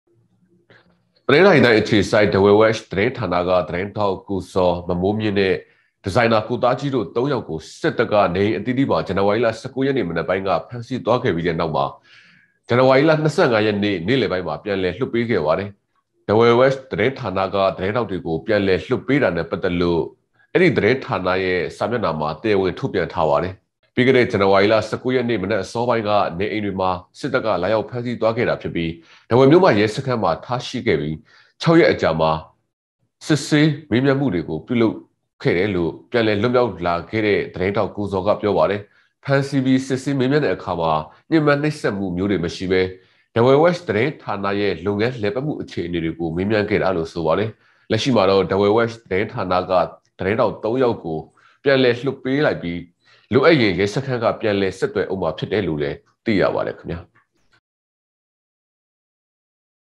ဖမ်းဆီးခံ Dawei Watch သတင်းဌာနက ၃ ဦး ပြန်လည်လွတ်မြောက် - DVB
တနင်္သာရီတိုင်းအခြေစိုက် Dawei Watch သတင်းဌာနက သတင်းထောက် ကိုဇော်၊ မမိုးမြင့်နဲ့ ဒီဇိုင်နာကိုသားကြီးတို ၃ ဦးကို စစ်တပ်က နေအိမ်အသီးသီးမှာ ဇန်နဝါရီ ၁၉ ရက်မနက်ပိုင်းက ဖမ်းဆီးသွားခဲ့ပြီးတဲ့နောက်  ဇန်နဝါရီ ၂၅ ရက် ဒီနေ့ မွန်းလွဲပိုင်းမှာ ပြန်လွှတ်ပေးလိုက်ပါတယ်။